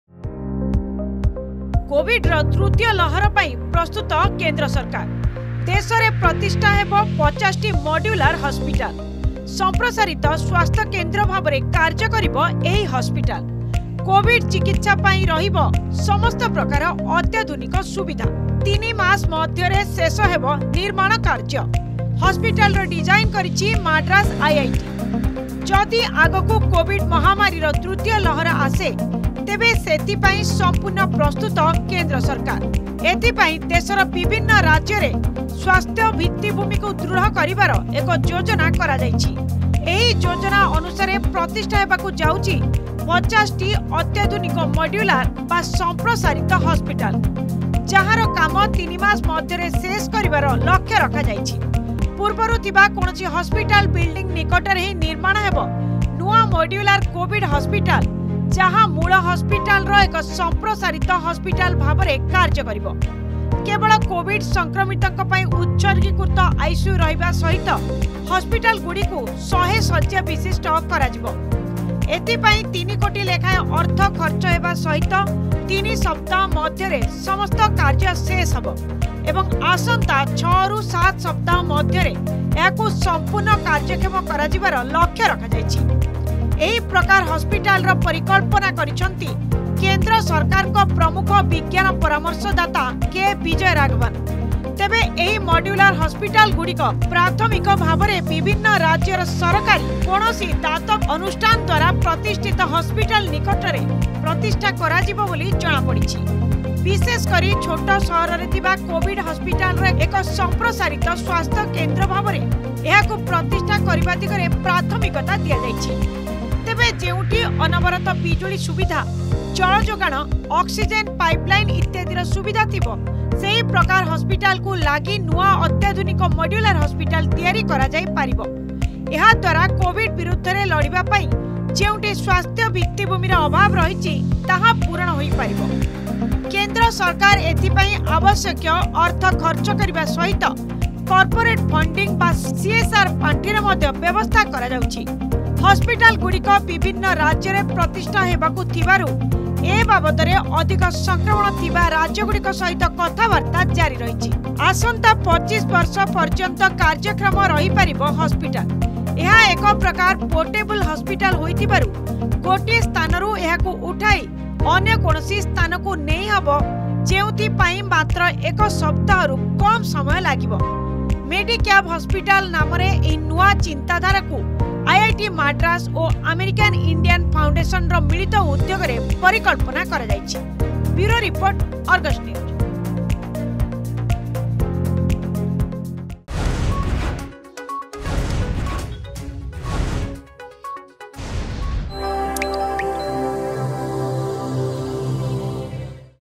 Center prepared for third wave of Covid-19 (କୋଭିଡର ତୃତୀୟ ଲହର ପାଇଁ ପ୍ରସ୍ତୁତ କେନ୍ଦ୍ର ସରକାର)
ଦେଶରେ ପ୍ରତିଷ୍ଠା ହେବ ୫୦ ଟି ମଡ୍ୟୁଲାର୍ ହସପିଟାଲ୍ I ସମ୍ପ୍ରସାରିତ ସ୍ବାସ୍ଥ୍ୟକେନ୍ଦ୍ର ଭାବରେ କାର୍ଯ୍ୟ କରିବ ଏହି ହସପିଟାଲ୍ I କୋଭିଡ୍ ଚିକିତ୍ସା ପାଇଁ ରହିବ ସମସ୍ତ ପ୍ରକାର ଅତ୍ୟାଧୁନିକ ସୁବିଧା I ୩ ମାସ ମଧ୍ୟରେ ଶେଷ ହେବ ନିର୍ମାଣ କାର୍ଯ୍ୟ I